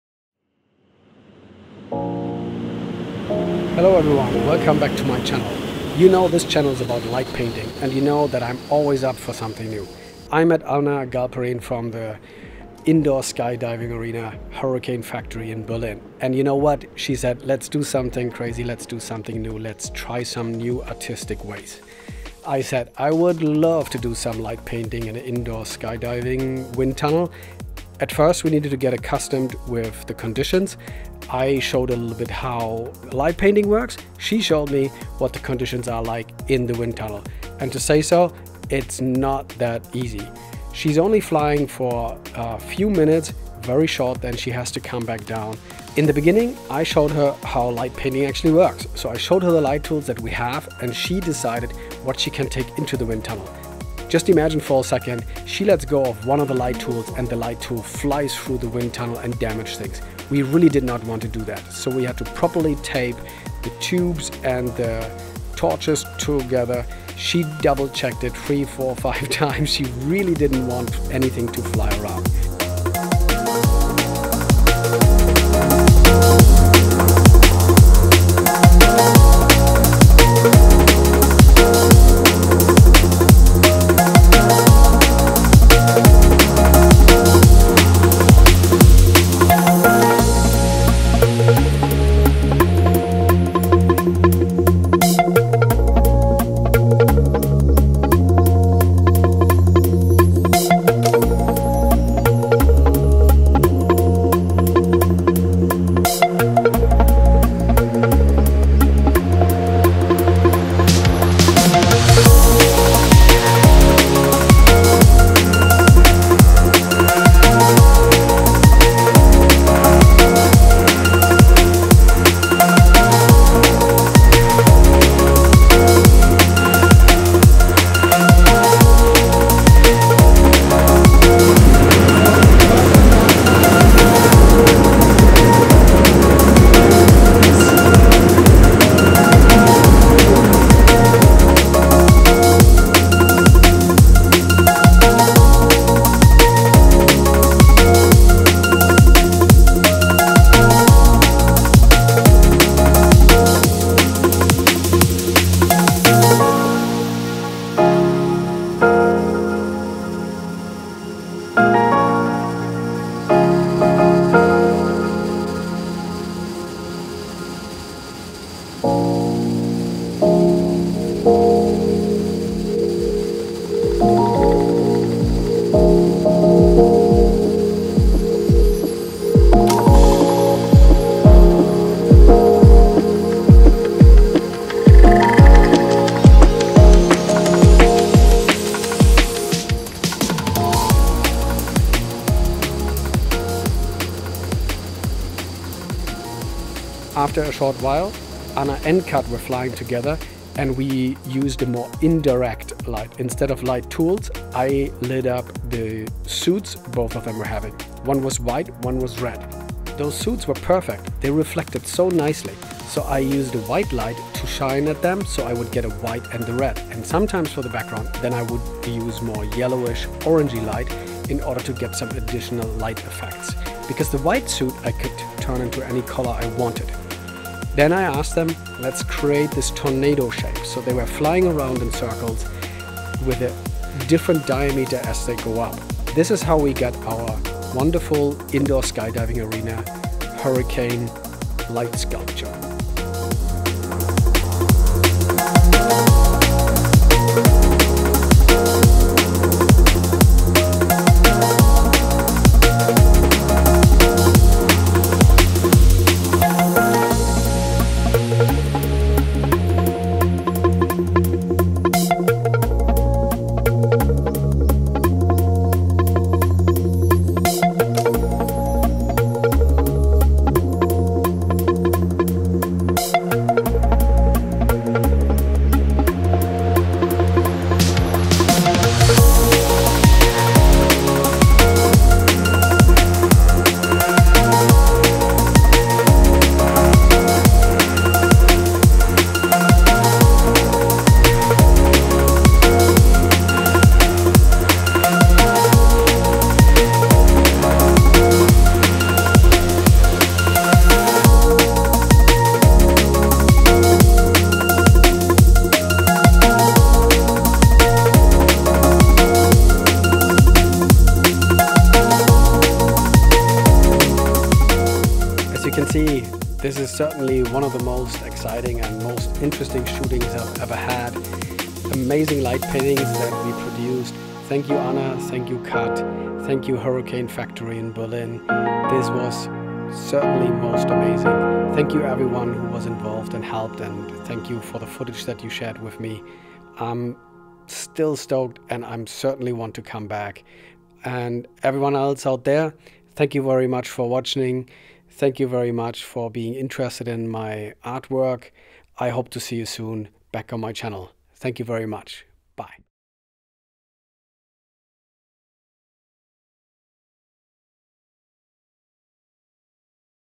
Indoor Sky Diving meets Light Painting
Over the years I got to light paint in very exotic places all around the world. The constant lookout for something new brought me together with Anna Galperin. She was about to gather artists for an Artist Night at the Hurrican Factory in Berlin. We decided together to test Light Painting in the Wind Tunnel.

Many thanks go out to the team of Hurricane Factory for letting me try my first ever Wind Tunnel Light Painting. Thank you Anna for organizing it.

HURRICANE FACTORY Berlin is the biggest wind tunnel in Germany. If you are on the brave end you can fly in Hurricane Factory with a wind speed of up to 270 km/h. You can book your flight time on their website super easily.